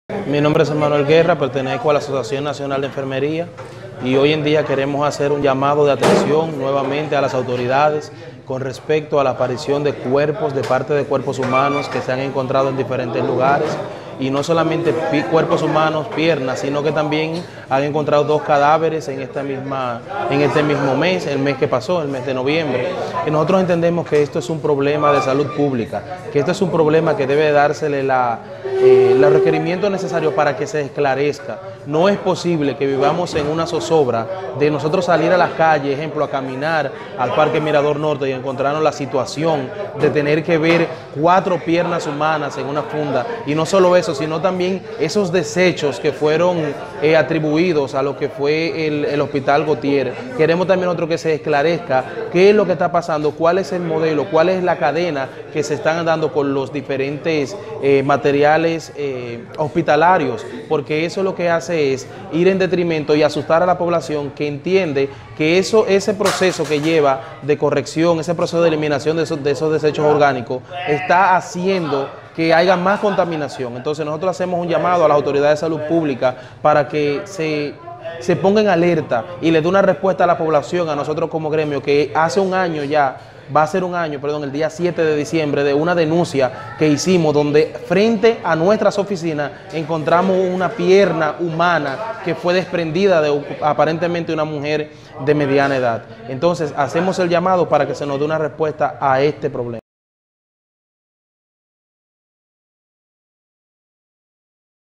Gremio de Enfermería exige respuesta sobre aparición de miembros humanos en espacios públicos
En nombre de la Asociación Nacional de Enfermería (ASONAEN), el licenciado Enmanuel Guerra, miembro de la Asociación Nacional de Enfermería, reiteró el llamado de atención a las autoridades de salud sobre la aparición de partes de cuerpos humanos y cadáveres en lugares públicos.
“Entendemos que esto es un problema de salud pública; es un problema al que debe dársele la relevancia necesaria para que se esclarezca. No es posible que vivamos en una zozobra de salir a la calle y encontrarnos la situación de tener que ver cuatro piernas humanas en una funda”, expresó el gremialista.